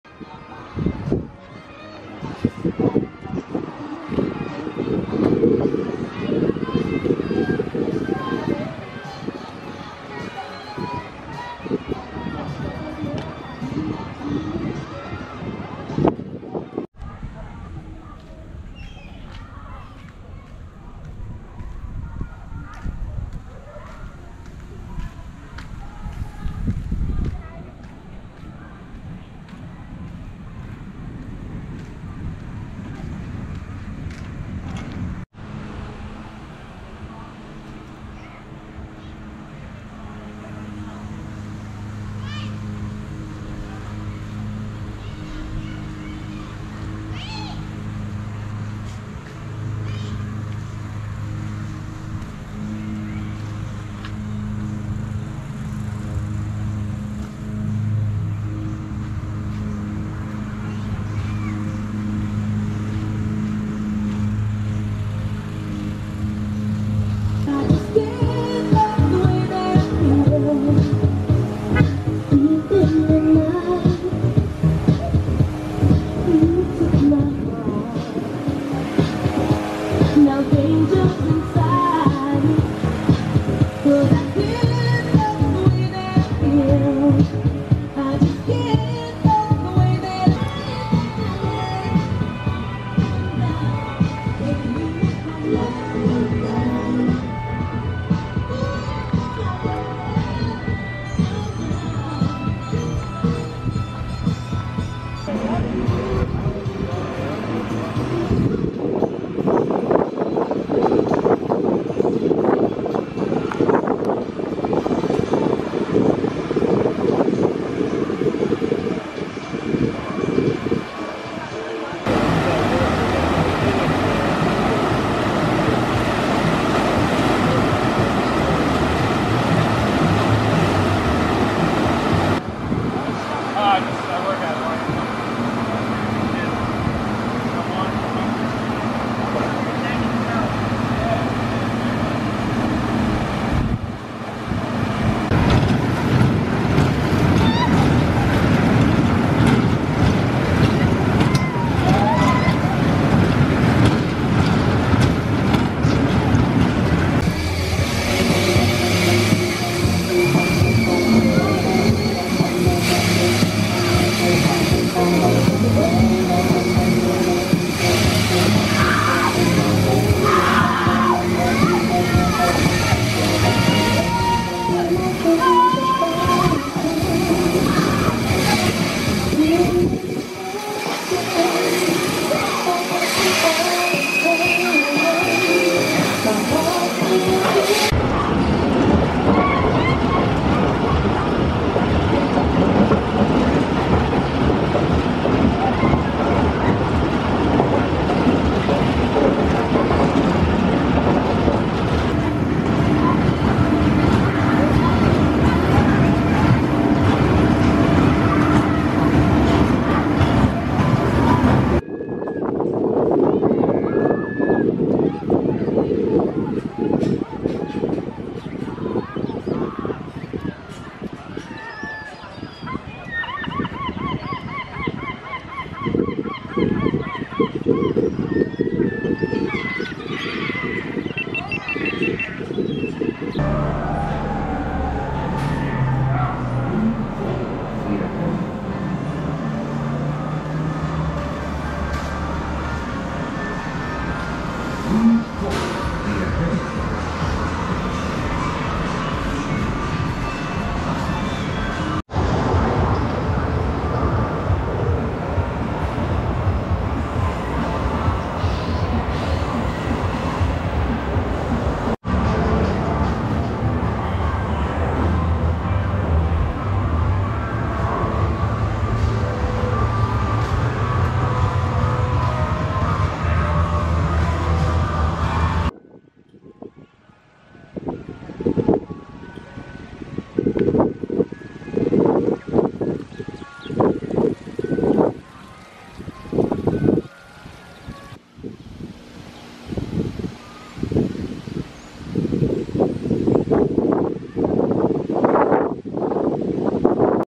Ocean Beach Park Off-Ride Footage, Connecticut Seaside Amusement Park | Non-Copyright
Ocean Beach Park is a popular beach in New London, Connecticut.  This complex features a large white sand beach plus an arcade, miniature golf, water slides, and a small amusement park.  The latter includes a mix of kiddie rides and adult flat rides plus a single roller coaster.

0:00- Parking Lot
0:17- Roadrunner Roller Coaster
0:41- Kiddie Land Walkthrough
1:45- Park Skyline
1:57- Waterslides
2:26- Scrambler (Eli Bridge Co.)
2:37- Roll-O-Plane (Eyerly)
3:08- Octopus (Spider)
3:22- Ferris Wheel (Eli Bridge Co.)
3:32- Beach
3:46- Boardwalk
3:54- Arcade
4:36- Mini Golf